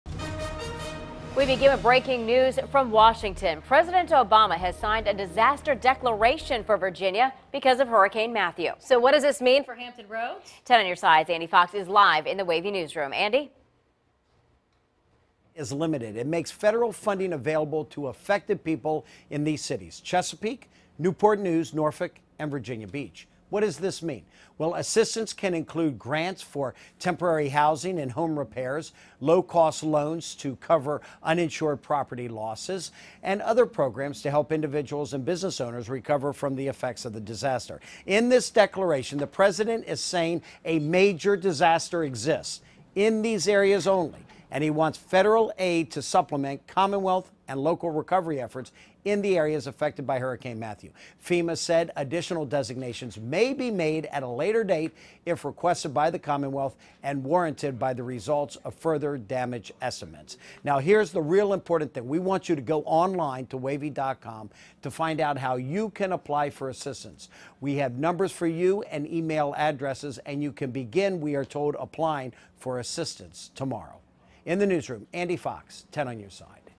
Pres. Obama signs Virginia disaster declaration for Hampton Roads cities
President Obama signed a Virginia disaster declaration Wednesday, which will provide supplemental federal aid for several Hampton Roads cities impacted by Hurricane Matthew.